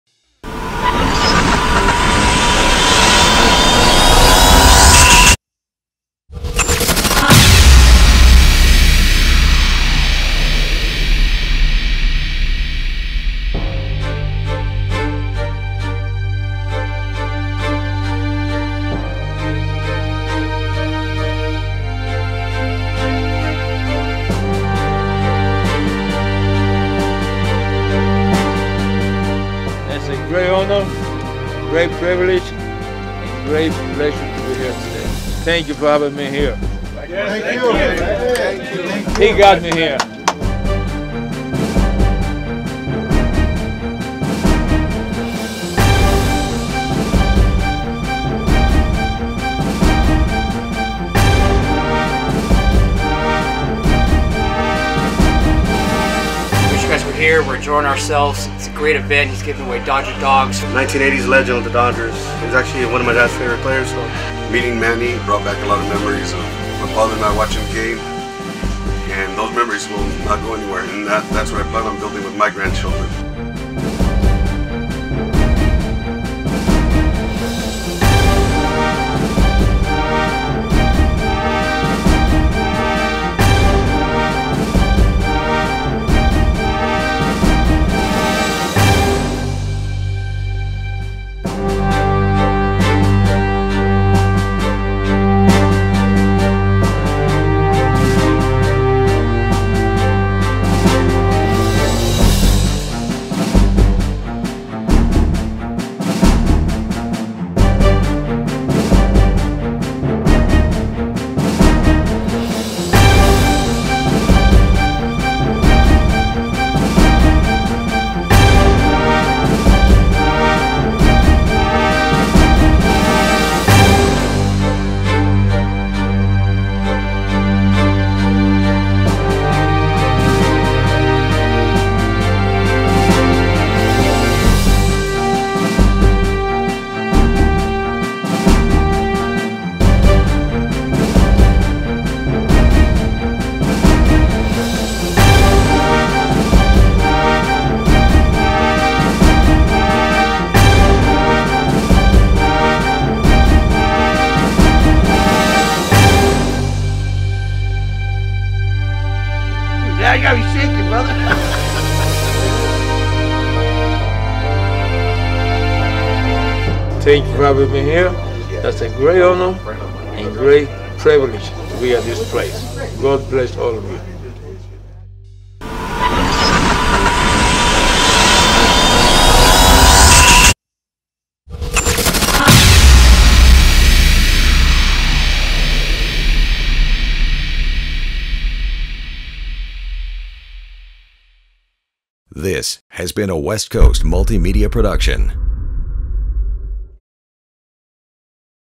Ultimate Pastime Sports Collectables Manny Mota Bobblehead Event
West Coast Multimedia was back at Ultimate Pastime Sports Collectables in Ontario for the Manny Mota Bobblehead Event April 22, 2017. Manny was amazing! A very kind man who was great with the fans especially the children! Thank You Manny!!!